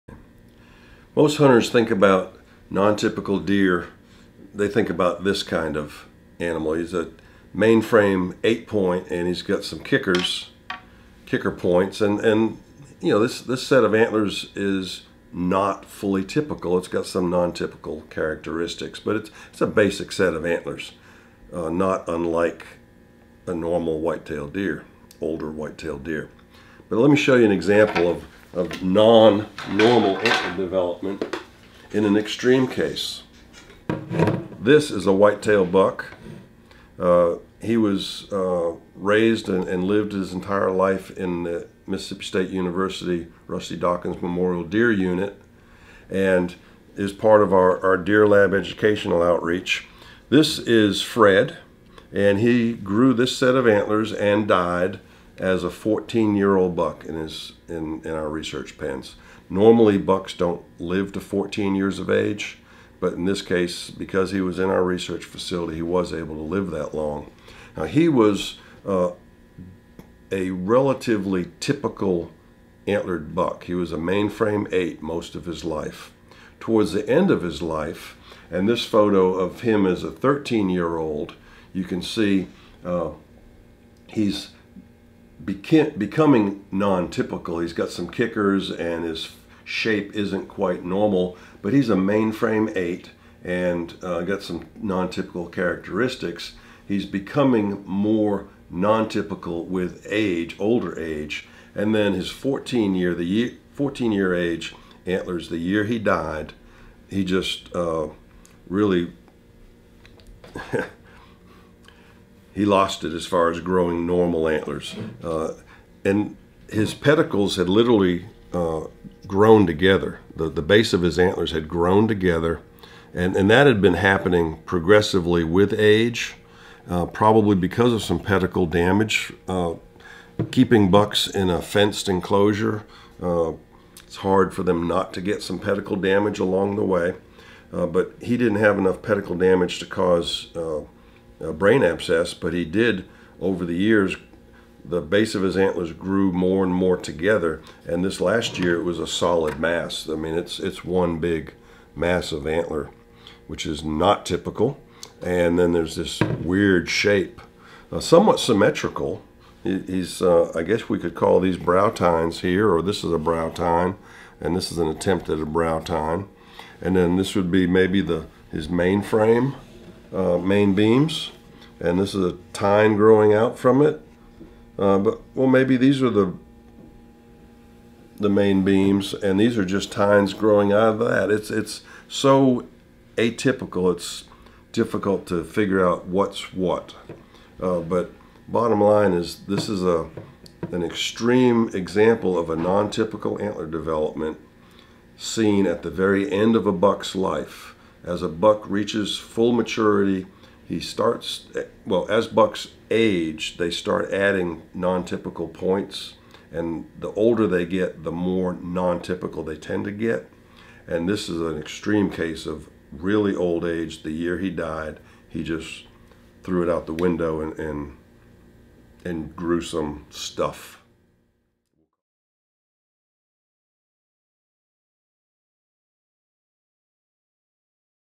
Antler Abnormalities In Old Deer
As bucks mature it is not uncommon for their antlers to begin exhibiting abnormalities. Here is an extreme example of abnormal antlers!